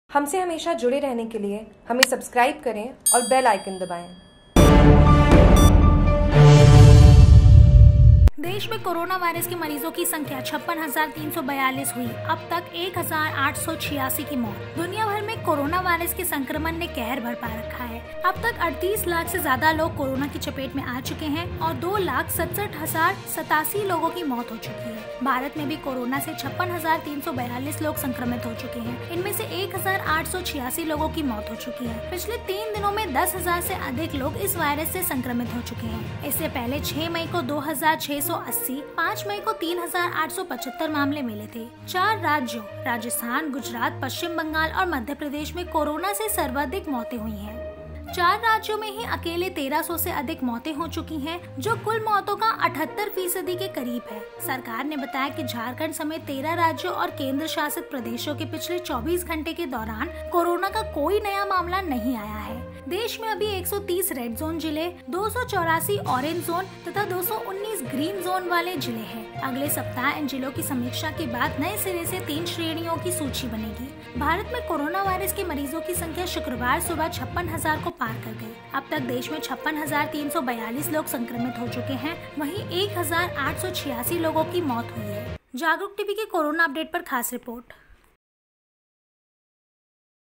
Jagruk TV CORONA UPDATE: देश में कोरोना वायरस के मरीजों की संख्या 56,342 हुई, अब तक 1,886 की मौत
Jagruk TV

देश में कोरोना वायरस के मरीजों की संख्या 56,342 हुई
अब तक 1,886 की मौत
नई दिल्ली  दुनियाभर में कोरोना वायरस के संक्रमण ने कहर बरपा रखा है। अब तक 38 लाख से ज्यादा लोग कोरोना की चपेट में आ चुके हैं और 2,67,087 लोगों की मौत हो चुकी है।
भारत में भी कोरोना से 56,342 लोग संक्रमित हो चुके हैं। इसमें से 1,886 लोगों की मौत हुई है। पिछले तीन दिनों में दस हजार से अधिक लोग इस वायरस से संक्रमित हो चुके हैं।
इससे पहले छह मई को 2680, पांच मई को 3875 मामले मिल थे। चार राज्यों महाराष्ट्र, गुजरात, पश्चिम बंगाल और मध्य प्रदेश में कोरोना से सर्वाधिक मौतें हुई हैं। चार राज्यों में ही अकेले 1300 से अधिक मौतें हो चुकी हैं जो कुल मौतों का 78 फीसदी के करीब है।

सरकार ने बताया कि झारखंड समेत 13 राज्यों और केंद्र शासित प्रदेशों में पिछले 24 घंटों के दौरान कोरोना का कोई नया मामला नहीं आया है। देश में अभी 130 रेडजोन जिले, 284 ऑरेंज जोन तथा 219 ग्रीन जोन वाले जिले हैं। अगले सप्ताह इन जिलों की समीक्षा के बाद नए सिरे से तीन श्रेणियों की सूची बनेगी।

- भारत में कोरोना वायरस के मरीजों की संख्या शुक्रवार सुबह 56 हजार को पार कर गई। अब तक देश में 56,342 लोग संक्रमित हो चुके हैं। वहीं, 1,886 लोगों की मौत हुई है।